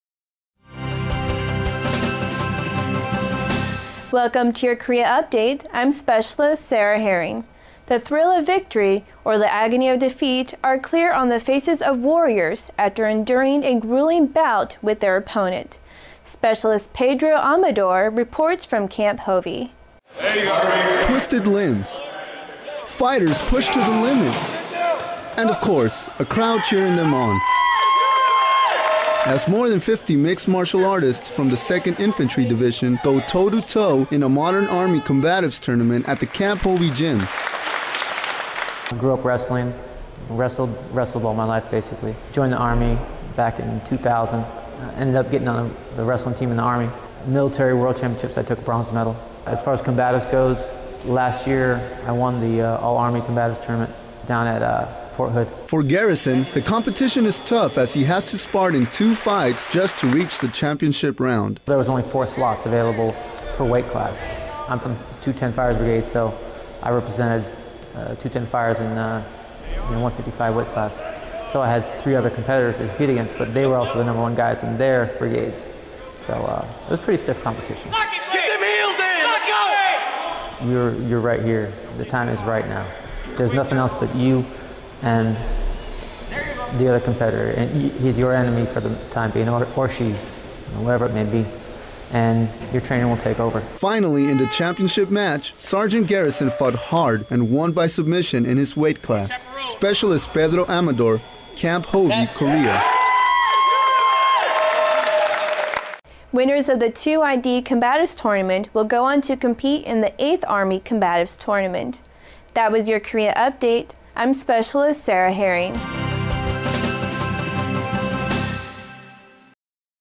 MilitaryMixedMartialArts.com - 2ID Combatives Tournament
- This edition features a story about the 2nd Infantry Division's Combatives Tournament.